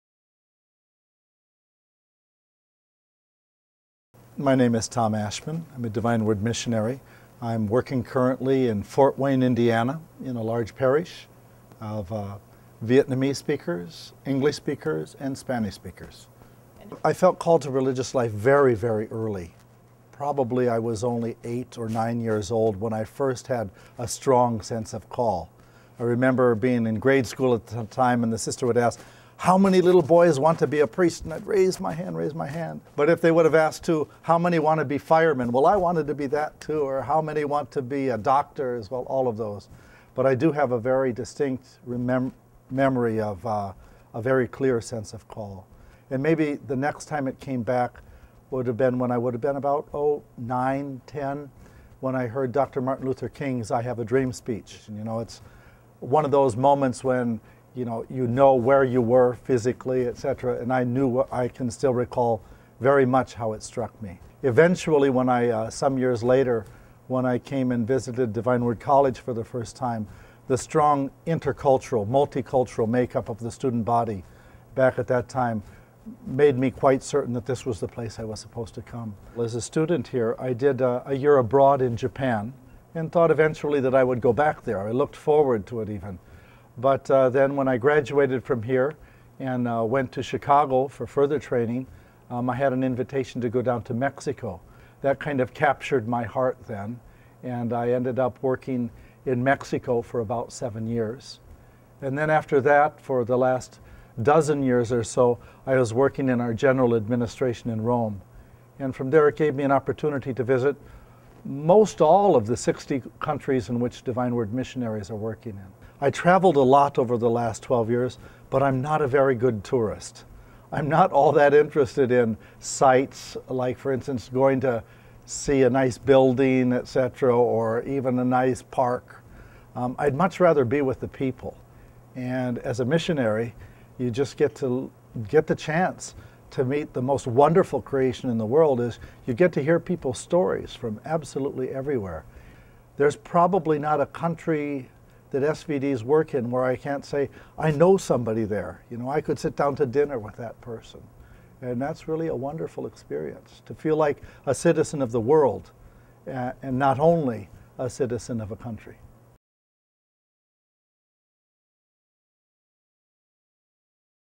My Call to Religious Life
Fr. Tom Ashemann talks about how and when he knew he was called to religious life.